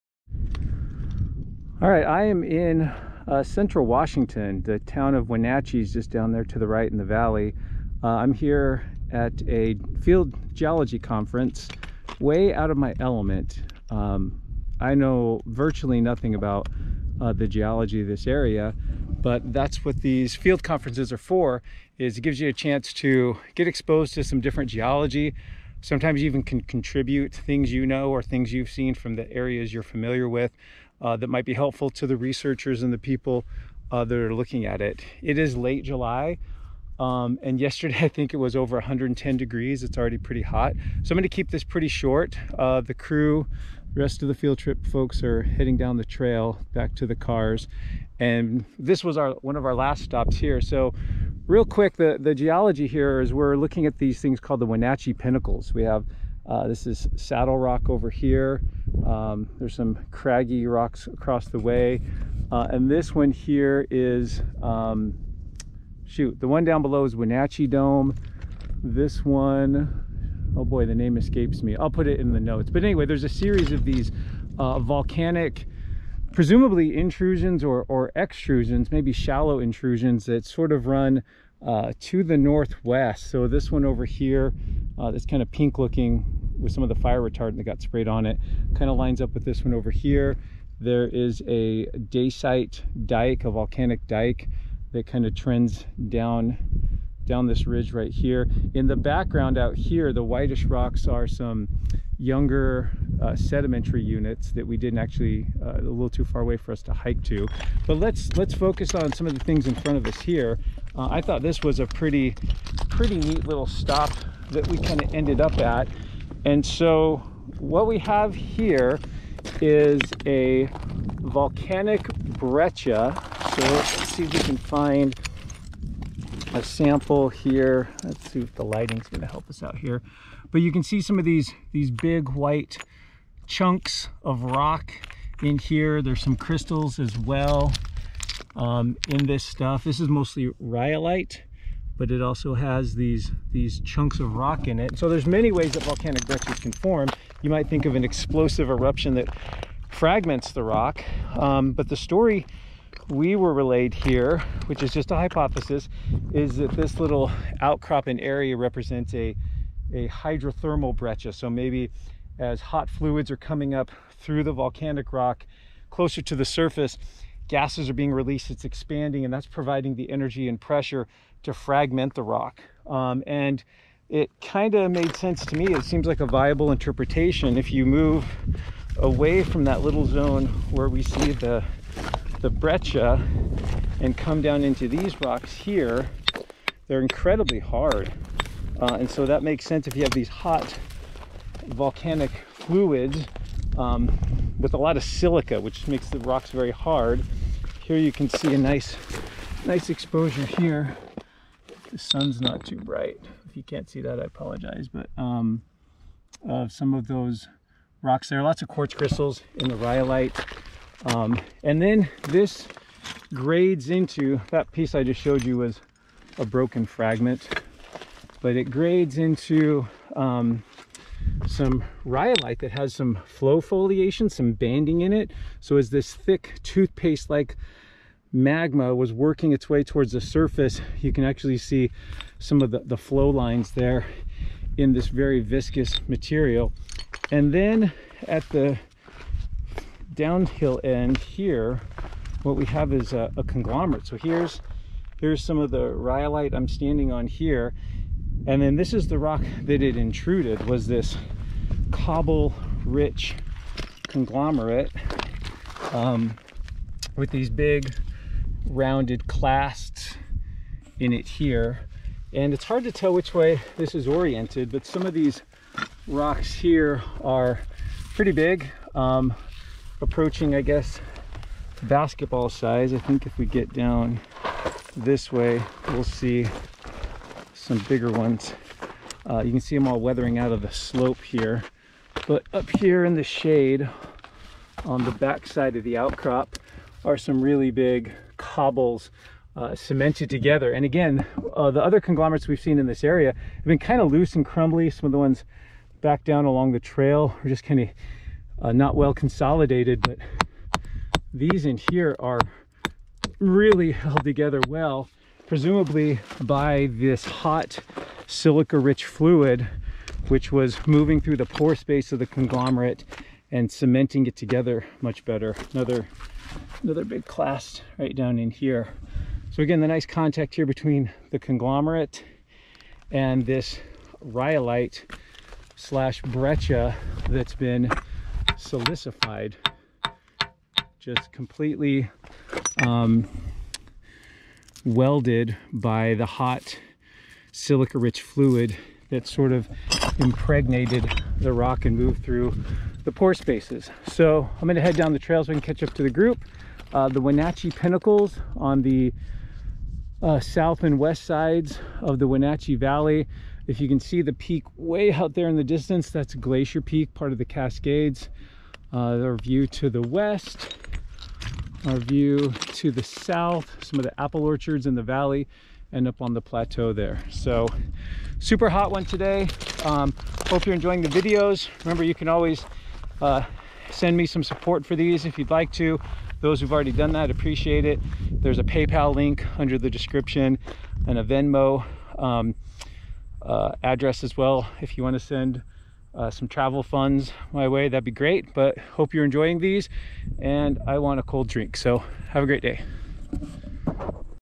Wenatchee Pinnacles: shallow magma intrusion, hydrothermal breccia, and silicified conglomerate
Jump right in at the tail end of a super hot half-day field trip, part of a geology field conference in Wenatchee, WA. Team up with geology professor Shawn Willsey as he learns some new stuff far from home. Here we will check out an instructive outcrop near the summit of Rooster Comb, one of the distinctive and prominent outcrops along the skyline southwest of Wenatchee. This series of northwest-trending domes, fins, pinnacles, and otherwise toothy rocks are composed of shallow intrusive rocks, mostly rhyolite here, and were emplaced about 45 million years ago, intruding slightly older sedimentary rocks.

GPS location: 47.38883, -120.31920

or click on the three dots below the viewer then "Thanks"

or a good ol' fashioned check to:
Shawn Willsey
148 Blue Lakes Blvd N